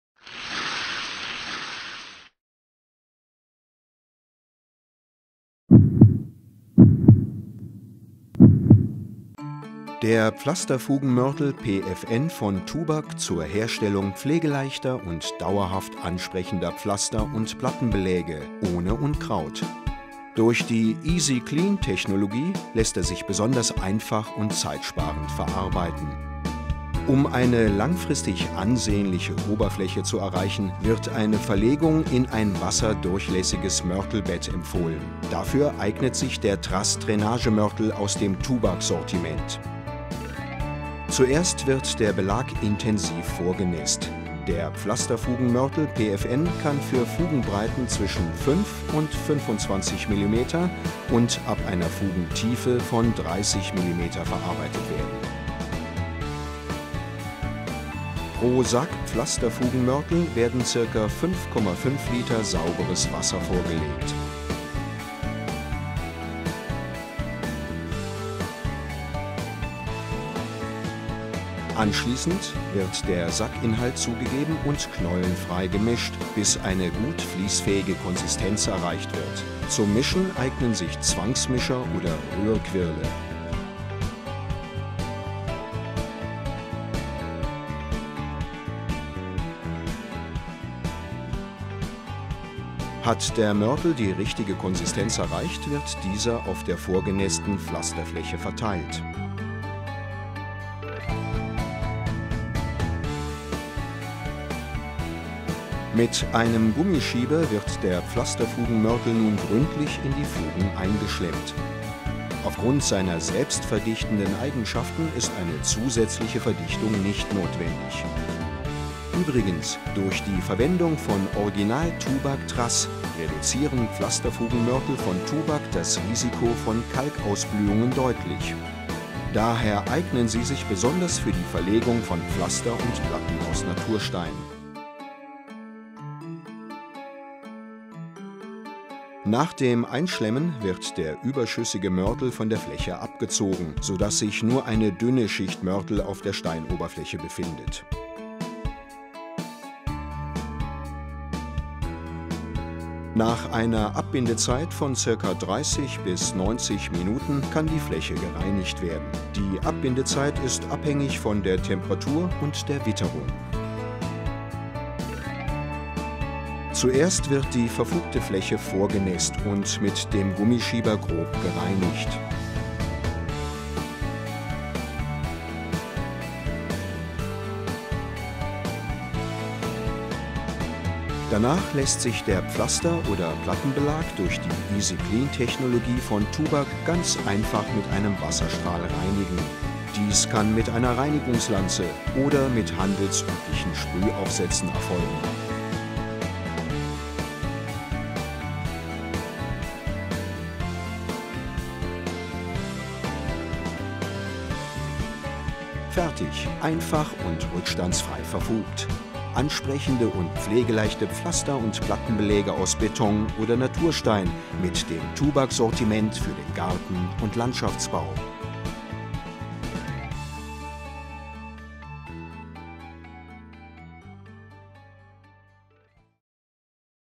Gartengestaltung mit dem wasserundurchlässigen tubag Pflasterfugenmörtel PFN
Mit dem wasserundurchlässigen Pflasterfugenmörtel PFN lassen sich Pflaster- und Plattenbeläge aus Beton oder Naturstein rückstandsfrei verfugen.
Durch die Easy Clean Technology von tubag können Oberflächen ganz einfach mit einem Wasserstrahl gereinigt werden.
Durch die Verwendung von original tubag Trass wird darüber hinaus das Risiko von Kalkausblühungen deutlich reduziert.